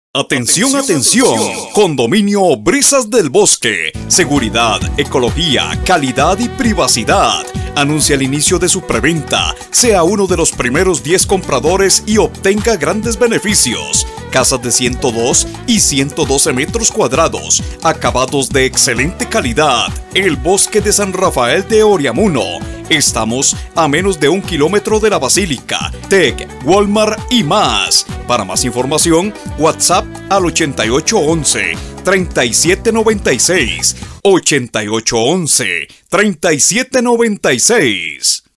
CONDOMINIO BRISAS DEL BOSQUE CARTAGO X PERIFONEO COREA
Atención, atención, CONDOMINIO BRISAS DEL BOSQUE; seguridad, ecología, calidad y privacidad; anuncia el inicio de su PREVENTA. Sea 1 de los primeros 10 compradores y obtenga grandes beneficios. Casas de 102 y 112 metros cuadrados. Acabados de excelente calidad. El Bosque de San Rafael de Oreamuno.
Estamos a menos de 1 kilómetro de la Basílica, TEC, Walmart y mas Para más información: Wathsapp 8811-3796